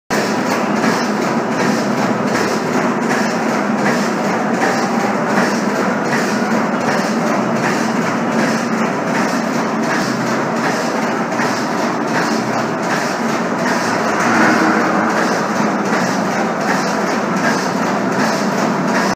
Mini-Mover LP Series Conveyor in Medical Packaging Line
Here's a  Mini-Mover LP Series Conveyor used in "squeeze: mode for package integrity testing on medical gel tubes. The tubes are being ejected from a tube filling machine.  The LP conveyor mounted over the conveying surface is set to squeeze each tube.  This tests each tube to verify a complete seal of the seam along the bottom of the tube.  Prior to this, an employee would have to squeeze each tube by hand.